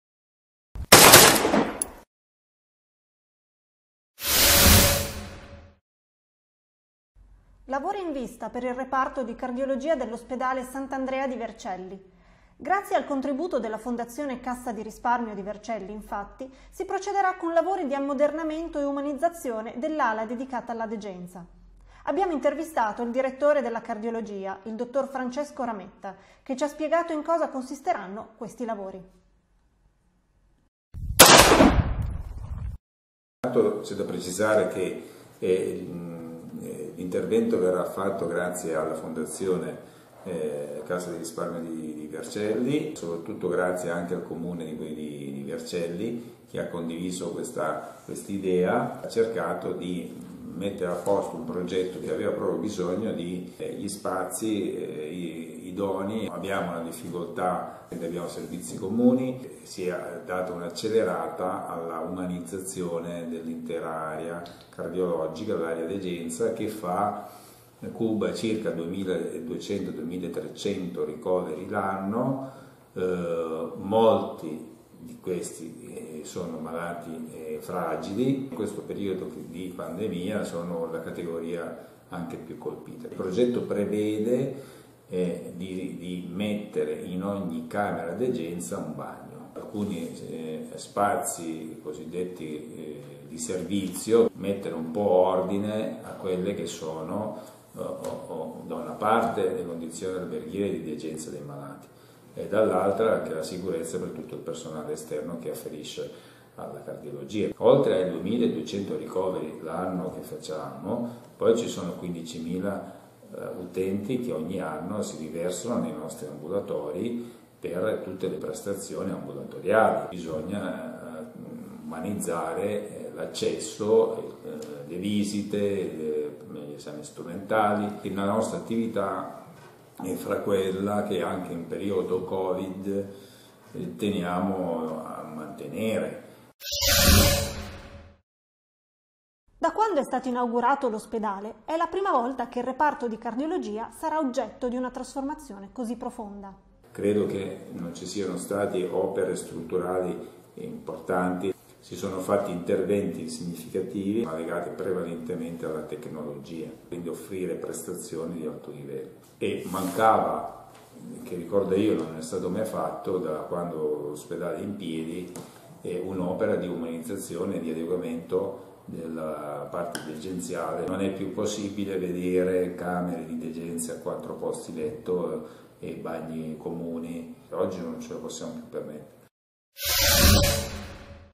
Ospedale di Vercelli, il reparto di Cardiologia cambia volto
Lavori in vista per il reparto di Cardiologia dell’ospedale Sant’Andrea di Vercelli. Grazie al contributo della Fondazione Cassa di Risparmio di Vercelli, infatti, si procederà con lavori di ammodernamento e umanizzazione dell’ala dedicata alla degenza. Abbiamo intervistato il direttore della Cardiologia, dott Francesco Rametta, che ci ha spiegato in cosa consisteranno.